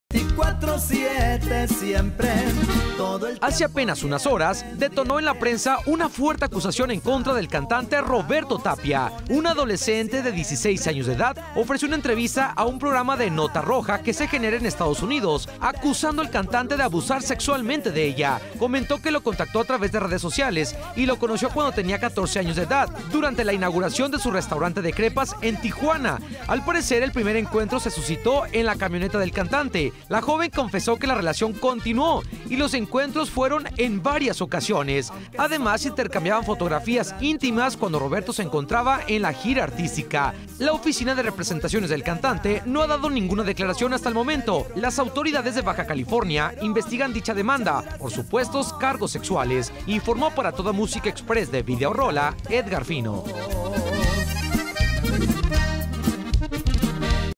*NOTA* ROBERTO TAPIA
Detona noticia en contra del Cantante Roberto Tapia